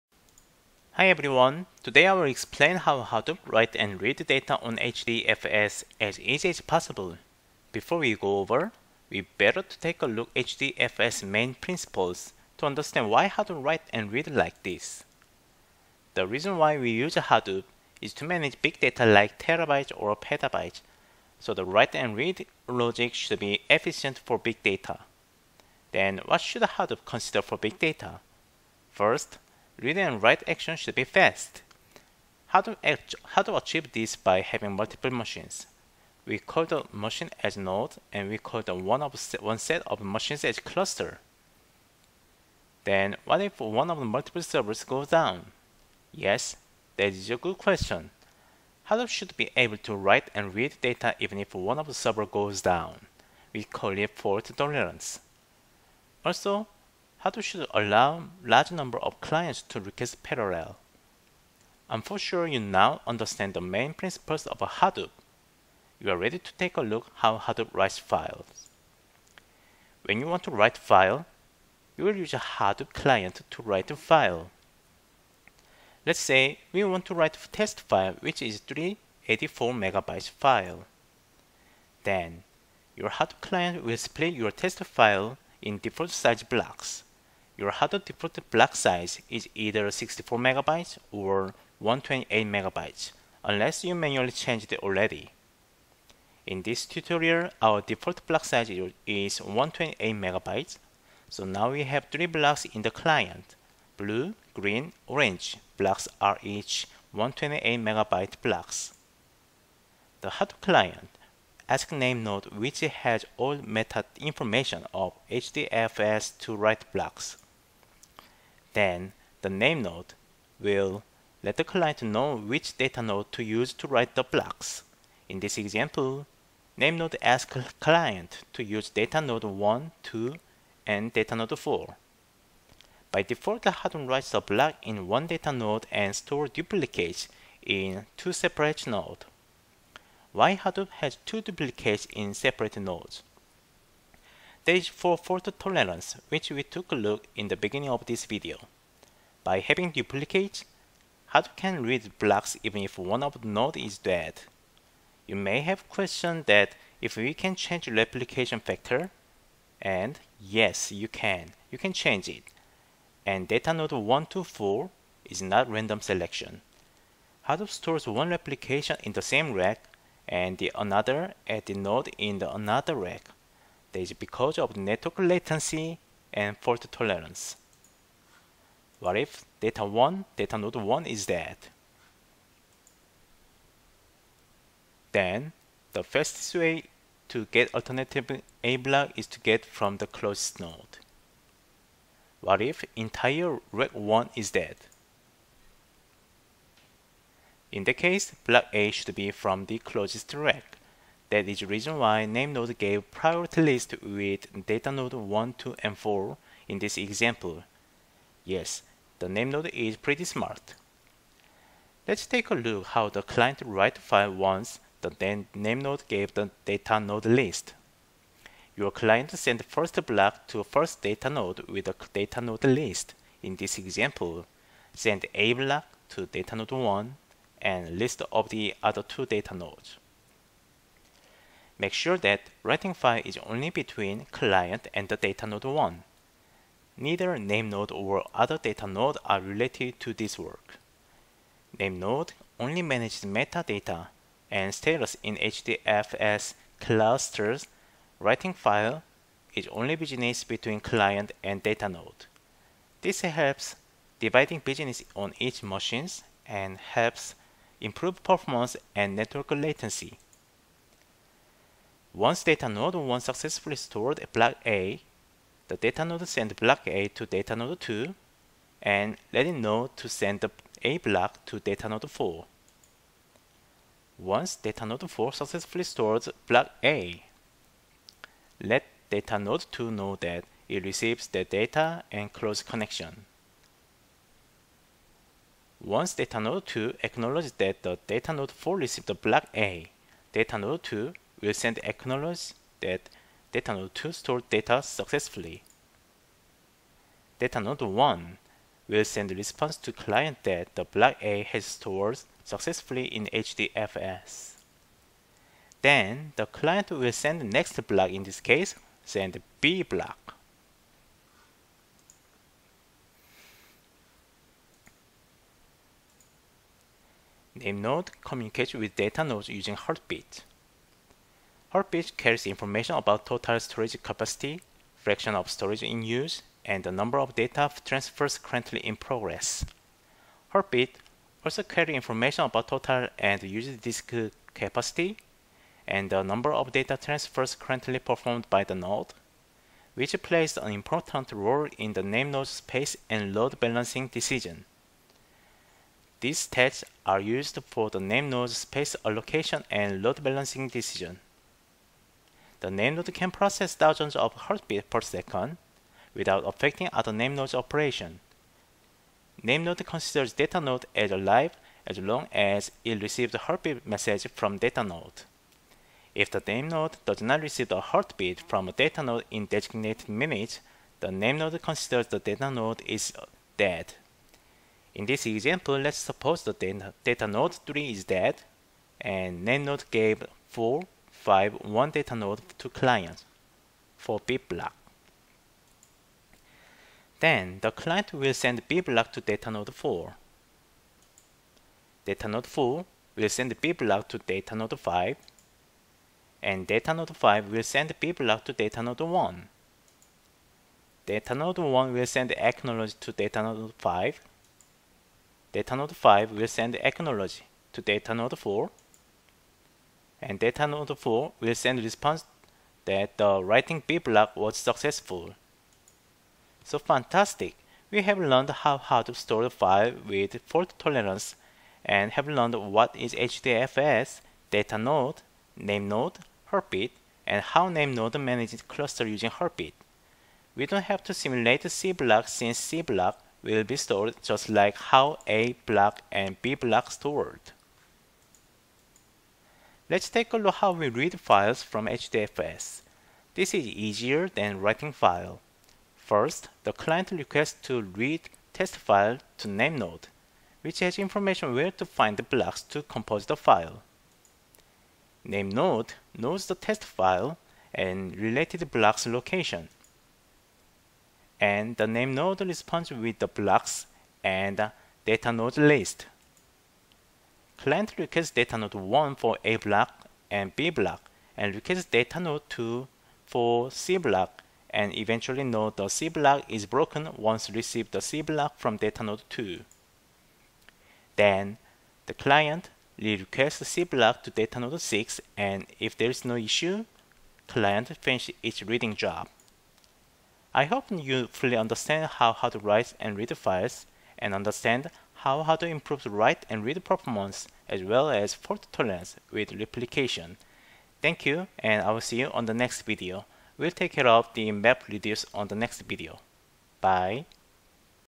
How Hadoop Distributed File System (HDFS) write and read file
in this tutorial, we will learn how HDFS write / read file and main principles of HDFS. you will learn key terminologies of HDFS auto-
magically during the tutorial.